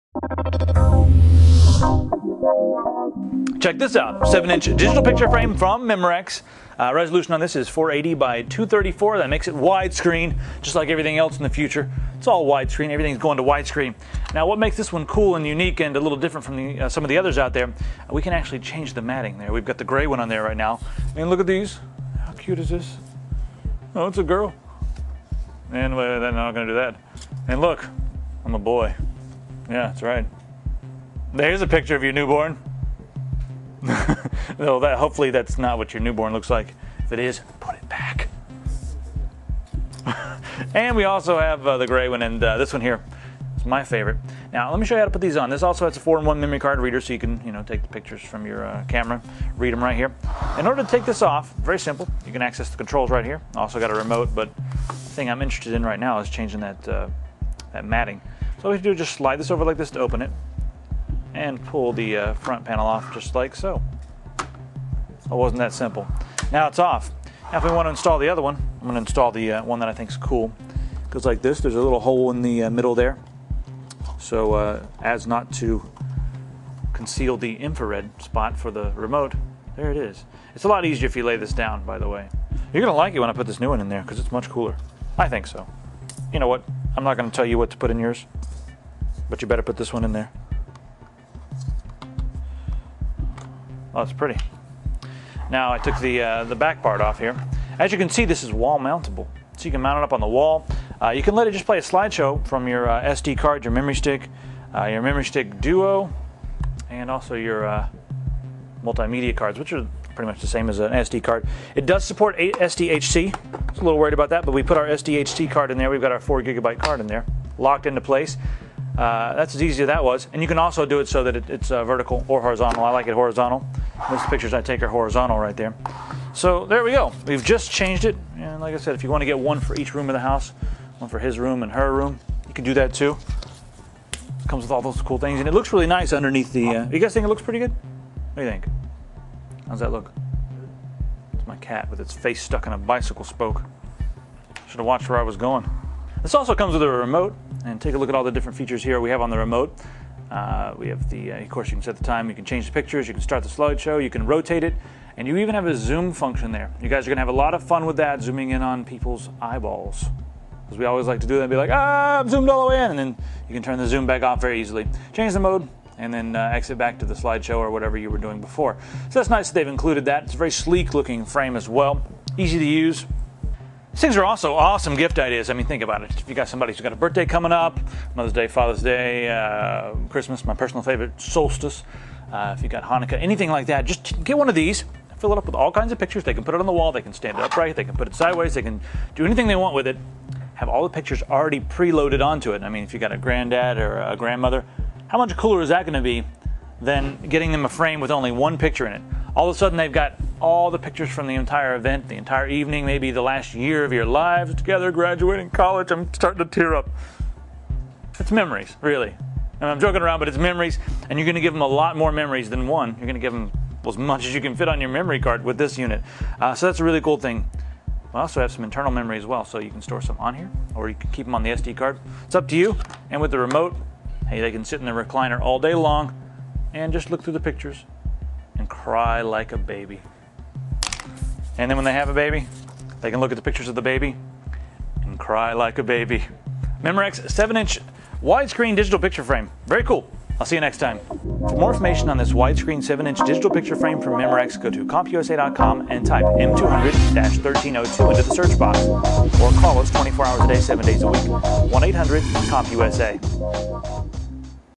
Memorex MDF0712-C 7" Widescreen Digital Photo Frame
Memorex Special Occasions Digital Photo Frame - Interchangeable for Life's Stages. The Memorex Special Occasions digital photo frame features a high-gloss, white finish, plus four interchangeable inserts - one with a silver metallic finish and three with wedding, baby boy and baby girl motifs. Viewing digital photos is fun, easy and entertaining with the frame's slideshow transition effects. The frame features a seven-inch widescreen LCD display and supports photos in JPEG format, with 480x234 screen resolution. Mom also will benefit from the ease of the included USB port and 2-in-1 memory card slot that's compatible with Secure Digital™ and MultiMedia Card™ formats. An AC power adapter is included.